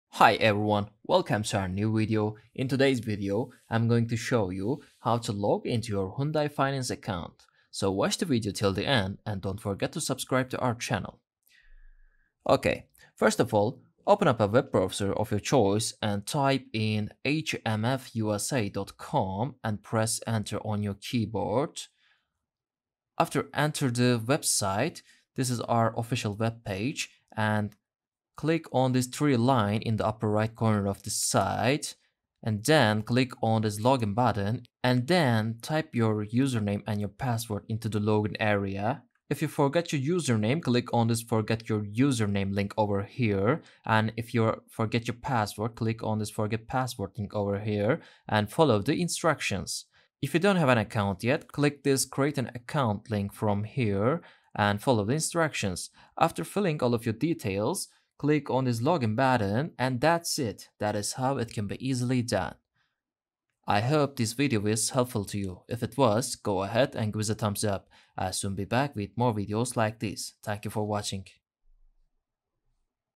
Hyundai Motor Finance Login: Login to Your Account ⏬👇
Logging into your Hyundai Motor Finance account involves a few straightforward steps that allow you to manage your auto loan or lease online. Here's a step-by-step guide to access your account:

Open your web browser and go to the Hyundai Motor Finance website. The exact URL can vary based on your country or region, but in the United States, it's typically
Locate the Login Section:

On the homepage, look for the "Log In" button or link. This is usually located at the top right corner of the page.
Enter Your Login Credentials:

Click on the "Log In" button to open the login page.
Enter your Username and Password in the respective fields. If you're a first-time user, you may need to register for online access first by clicking on a “Register” or “Sign Up” link and following the prompts to create your account.
Log In to Your Account:

After entering your credentials, click the “Log In” button to access your account dashboard.
Troubleshooting Login Issues:

If you encounter any issues logging in, such as forgetting your username or password, look for a link that says "Forgot Username" or "Forgot Password" on the login page. Click on the appropriate link and follow the instructions to retrieve or reset your login details.
Navigate Your Account:

Once logged in, you can view your account details, make payments, update personal information, and manage your auto loan or lease.
Log Out:

When you’re finished, ensure you log out of your account, especially if you’re using a public or shared computer, to protect your personal information.